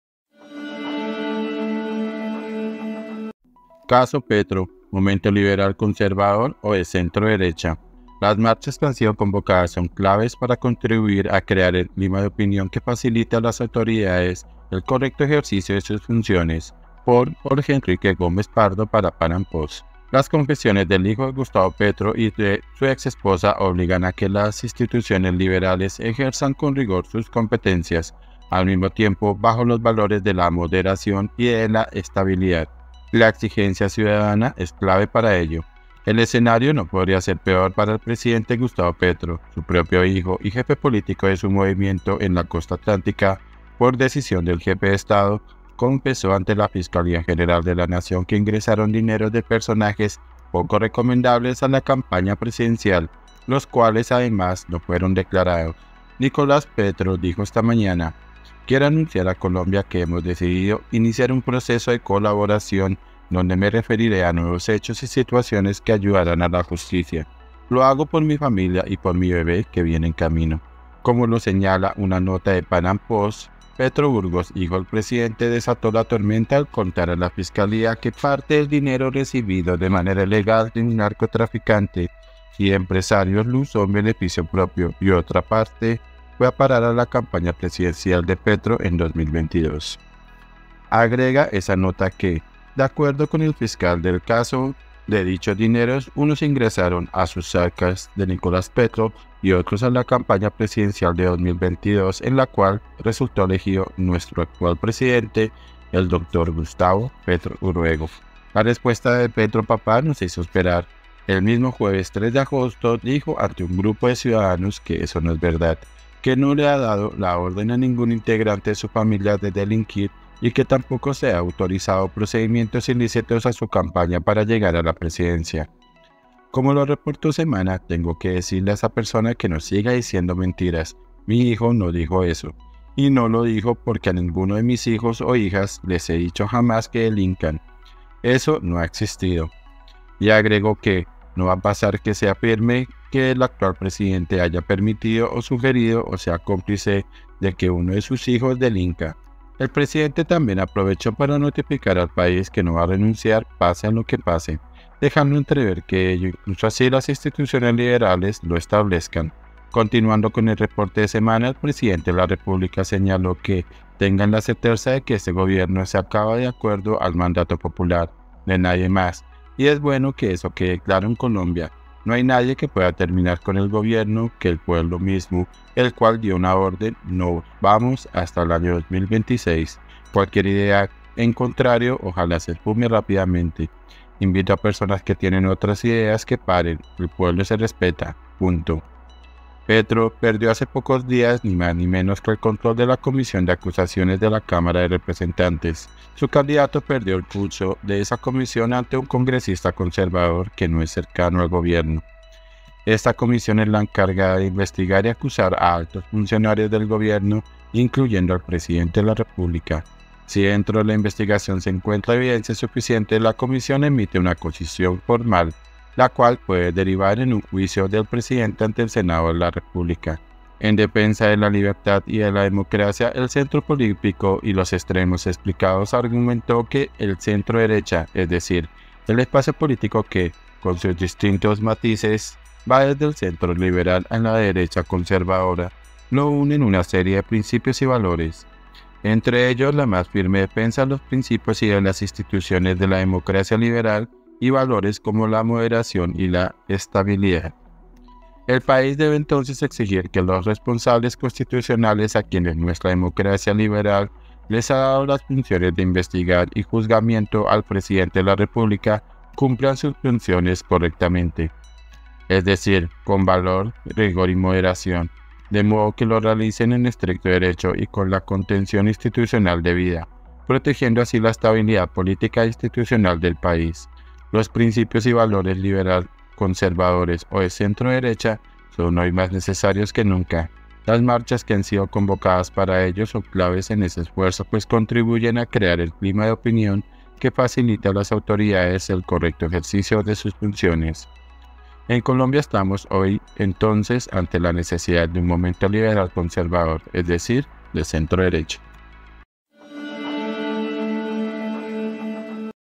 📢 CASO PETRO: MOMENTO LIBERAL CONSERVADOR O DE CENTRO-DERECHA
Las marchas que han sido convocadas son claves para contribuir a crear el clima de opinión que facilite a las autoridades el correcto ejercicio de sus funciones.
columna JORGE ENRIQUE GÓMEZ PARDO